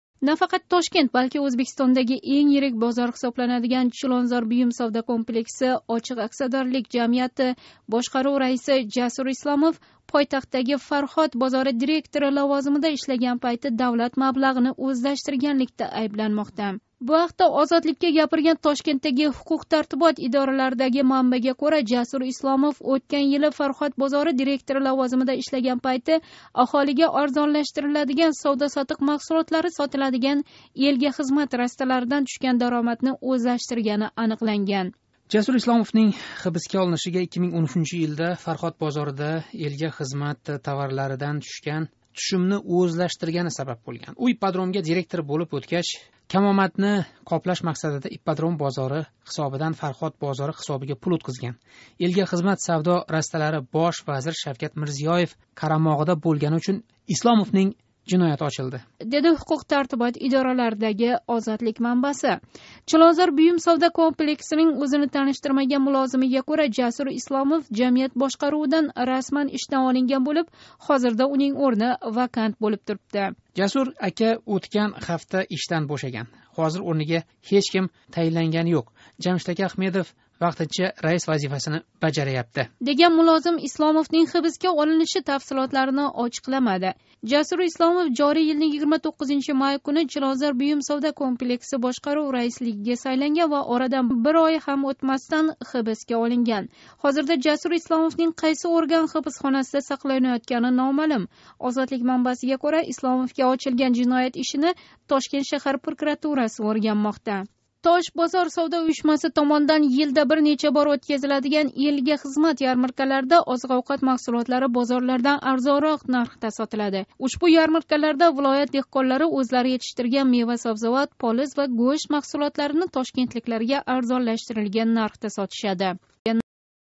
Ипподром раиси "Элга хизмат" ярмаркасидан пул ўғирлашда гумон қилинмоқда
Тошкентдаги энг йирик "Чилонзор буюм савдо комплекси" (Ипподром бозори) бошқарув кенгаши раиси Жасур Исломов давлат маблағини ўзлаштиришга оид бир қатор молиявий жиноятларда гумон қилинмоқда. Тошкентдаги ҳуқуқ-тартибот идораларидан олинган маълумотга кўра, Жасур Исломов ўтган ҳафта ҳибсга олинган.

Нафақат Тошкент балки Ўзбекистондаги энг йирик бозор ҳисобланадиган "Чилонзор буюм савдо комплекси" очиқ акциядорлик жамиятига атига бир ой раислик қилган Жасур Исломов давлат маблағини ноқонуний ўзлаштирганлик ва бозор маблағини суистеъмол қилганликда гумон қилинмоқда.

Тошкент ҳуқуқ-тартибот идораларидаги Озодлик манбасига кўра, Жасур Исломов ўтган йили "Фарҳод" бозори директори лавозимида ишлаган пайти аҳолига арзон нархдаги озиқ-овқат маҳсулотлари сотиладиган "Элга хизмат" ярмаркасидан тушган даромадни ўзлаштиргани аниқланган: